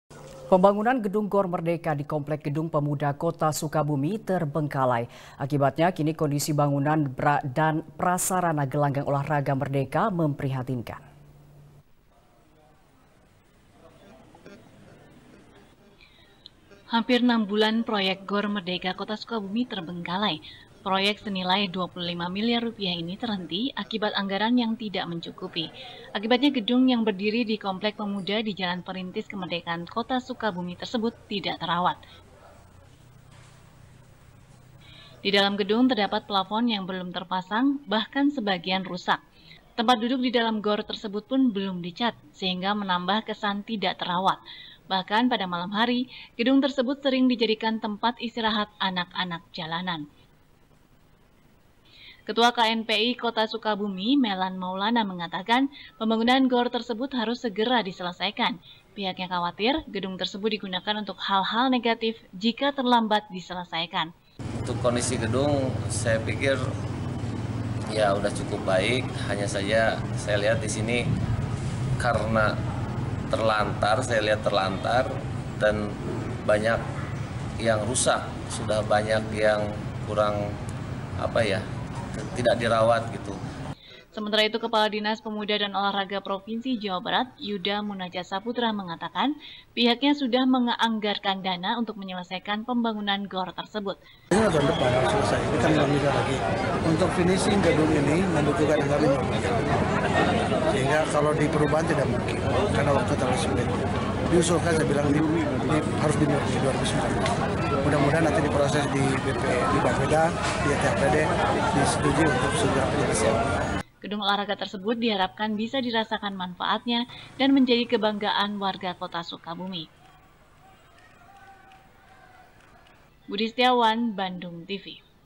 Gor mereka kota sukabumi terbengkalai | SBR | BANDUNGTV
SBR | BANDUNGTV | IWAN SEPPRIADI | Pembangunan gedung gor merdeka di komplek gedung pemuda kota sukabumi, terbengkalai. akibatnya, kini kondisi bangunan dan prasarana gelanggang olah raga merdeka, memprihatinkan.